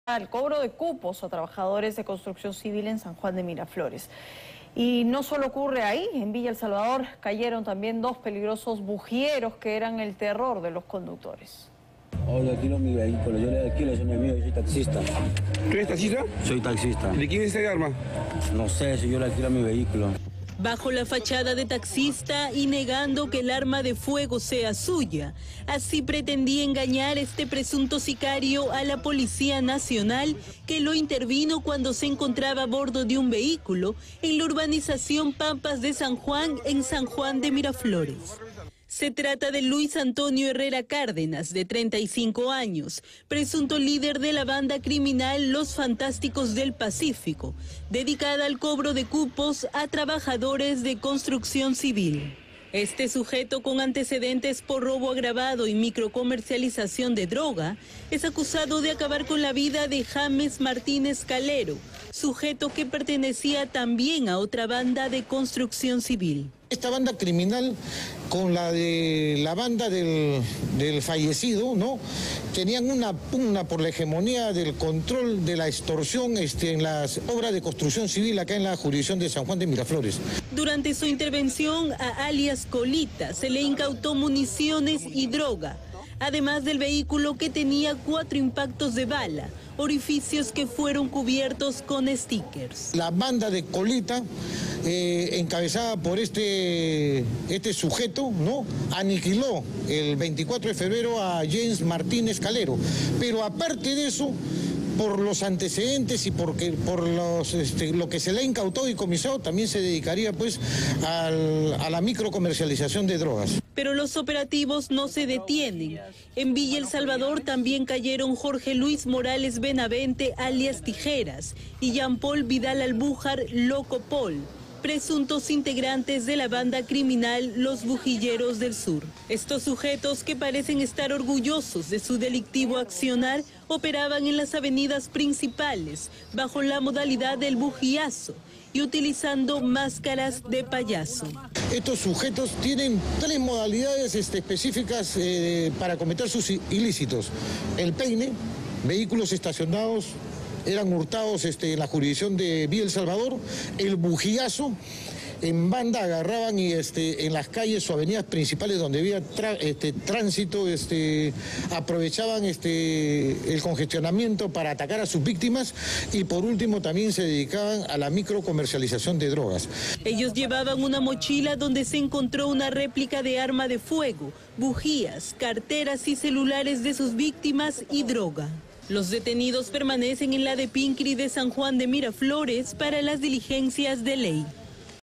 Cae líder de "Los Fantásticos del Pacifico" en SJM: decía ser taxista pero cobraba cupos a obreros
Bajo la fachada de taxista y negando la propiedad del arma de fuego encontrada, Luis Antonio Herrera Cárdenas, de 35 años, presunto líder de la banda criminal "Los Fantásticos del Pacífico", fue intervenido por la policía en la urbanización Pampas de San Juan, en San Juan de Miraflores. Esta agrupación delictiva se dedicada al cobro de cupos a trabajadores de construcción civil.

Herrera Cárdenas, quien tiene antecedentes por robo agravado y microcomercialización de droga, es acusado de acabar con la vida de James Martínez Calero, miembro de otra banda de construcción civil. Ambas bandas tenían una pugna por el control de la extorsión en obras del distrito.

Durante la intervención, se le incautó municiones y droga, además de un vehículo con cuatro impactos de bala cubiertos con stickers. La banda liderada por Herrera Cárdenas habría sido responsable del homicidio de Martínez Calero el 24 de febrero. Además, se le atribuye la microcomercialización de drogas.

En otro operativo en Villa El Salvador, fueron capturados Jorge Luis Morales Benavente, alias "Tijeras", y Jean Paul Vidal Albujar, alias "Loco Paul", presuntos integrantes de la banda criminal "Los Bujilleros del Sur". Estos individuos, que operaban bajo la modalidad del "bujiazo" con máscaras de payaso, tenían tres modalidades específicas para cometer sus ilícitos: el "peine", el "bujo en banda", y el ataque en las calles y avenidas principales. También se les incautó una réplica de arma de fuego, carteras, celulares de sus víctimas y droga.

Todos los detenidos permanecen en la comisaría Depincri de San Juan de Miraflores para las diligencias de ley.